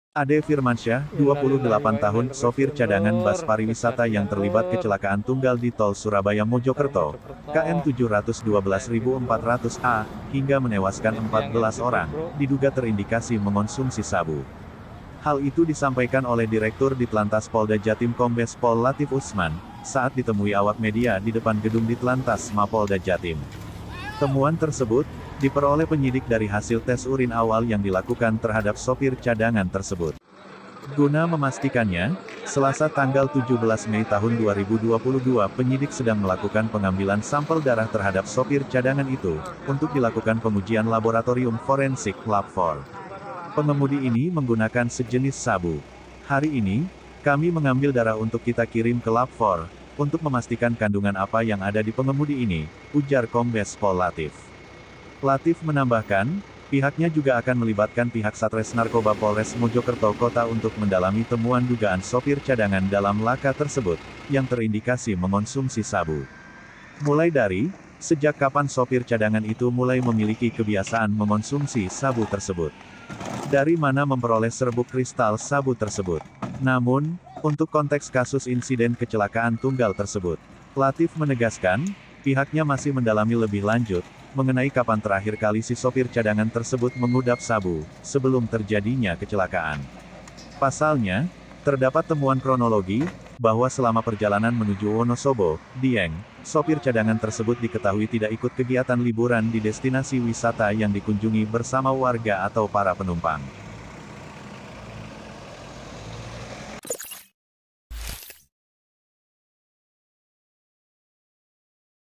Sopir Cadangan Bus Maut di Tol Surabaya Mojokerto Diduga Konsumsi Sabu
SURYA.CO.ID, SURABAYA - Ade Firmansyah (28) sopir cadangan bus pariwisata yang terlibat kecelakaan tunggal di Tol Surabaya-Mojokerto, KM 712.400/A, hingga menewaskan 14 orang, diduga terindikasi mengonsumsi sabu.

Hal itu disampaikan Direktur Ditlantas Polda Jatim Kombes Pol Latif Usman, saat ditemui awak media di depan Gedung Ditlantas Mapolda Jatim, Selasa (17/5/2022).

Video : rekaman warga / zaq
Reporter : Luhur Pambudi